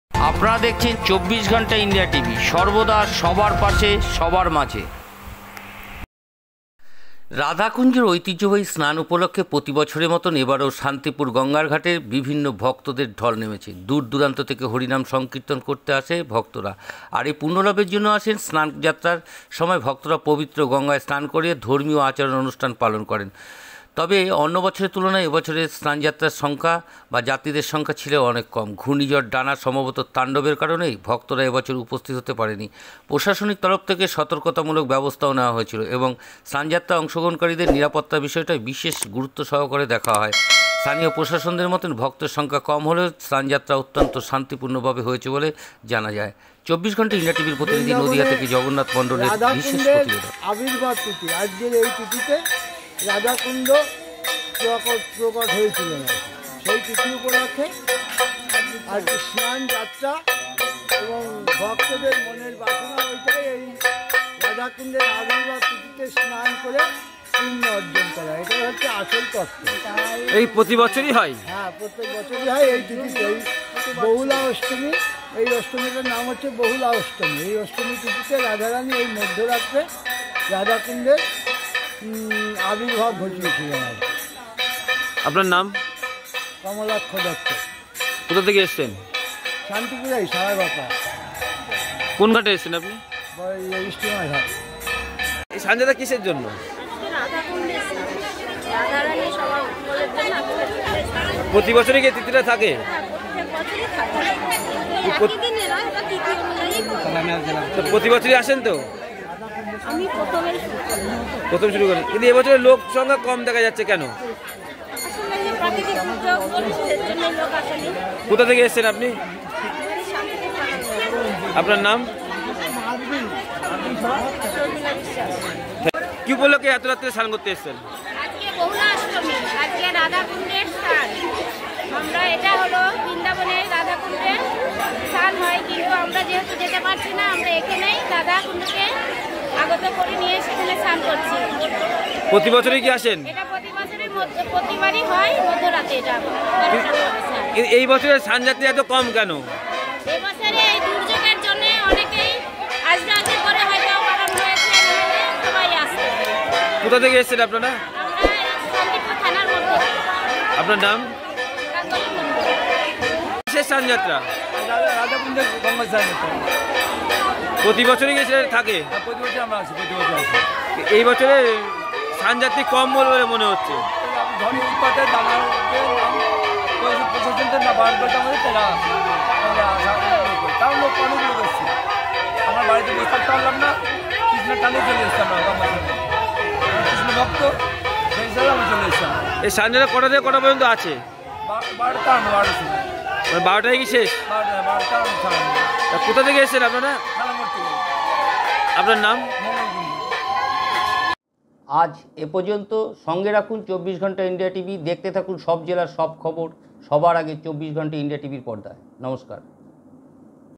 24ghantaindiatv শান্তিপুরের গঙ্গার বিভিন্ন ঘাটে ভক্তদের ঢল নেমেছে
24ghantaindiatv রাধাকুঞ্জের ঐতিহ্যবাহী স্নান যাত্রা উপলক্ষে প্রতি বছরের মতো এবারও শান্তিপুরের গঙ্গার বিভিন্ন ঘাটে ভক্তদের ঢল নেমেছে। দূর-দূরান্ত থেকে হরিনাম সংকীর্তন করতে করতে ভক্তরা এই পূর্ণলাভের জন্য আসেন। স্নান যাত্রার সময় ভক্তরা পবিত্র গঙ্গায় স্নান করে ধর্মীয় আচার-অনুষ্ঠান পালন করেন।তবে অন্যান্য বছরের তুলনায় এবছর স্নান যাত্রীদের সংখ্যা ছিল অনেক কম। ঘূর্ণিঝড় "ডানা"র সম্ভাব্য তাণ্ডবের কারণে বহু ভক্তই এবছর উপস্থিত হতে পারেননি। প্রশাসনের তরফ থেকে সতর্কতামূলক ব্যবস্থা নেওয়া হয়েছিল, এবং স্নান যাত্রায় অংশগ্রহণকারীদের নিরাপত্তার বিষয়টি বিশেষ গুরুত্ব সহকারে দেখা হয়।স্থানীয় প্রশাসনের মতে, ভক্তদের সংখ্যা কম হলেও যাত্রা অত্যন্ত শান্তিপূর্ণভাবে সম্পন্ন হয়েছে।